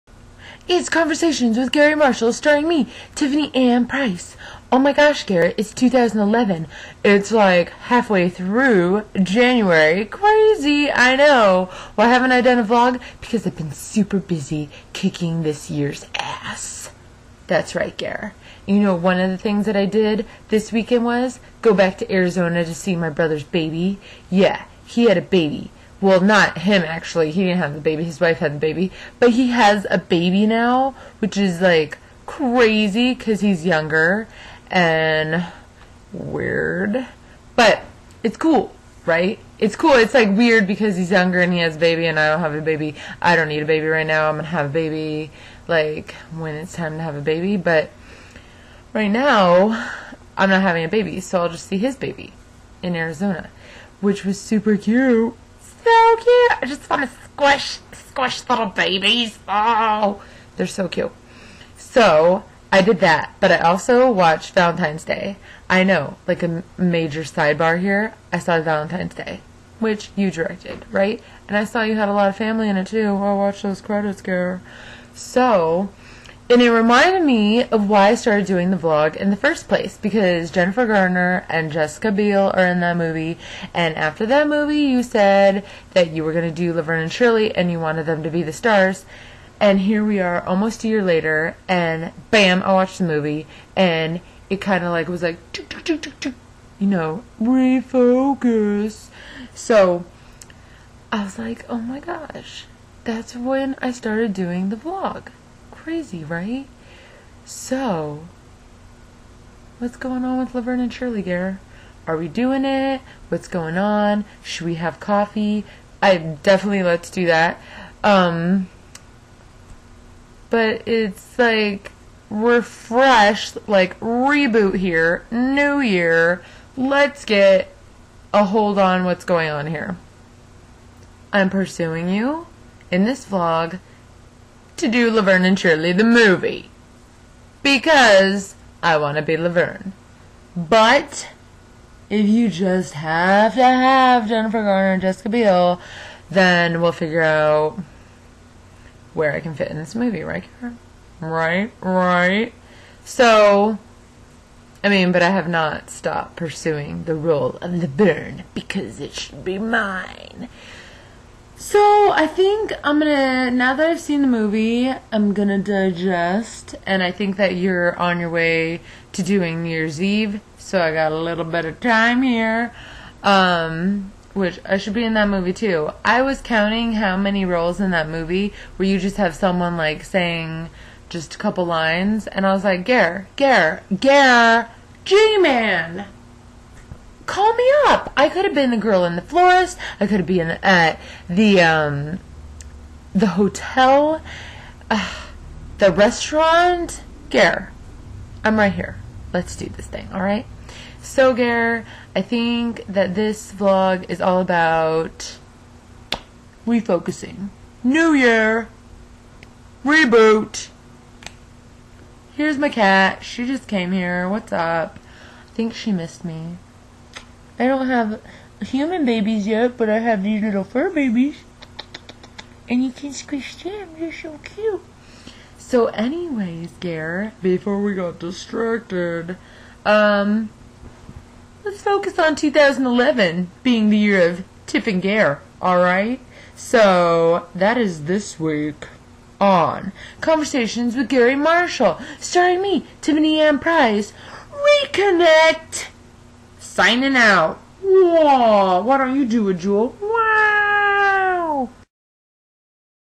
Conversations with Garry Marshall #37
G-man! Over the weekend I watched one of your films and it reminded me of the whole reason why I started up this vloggy vlog. It's because you mentioned doing Laverne and Shirley the movie. What happened with that Gar? Time to refocus!!!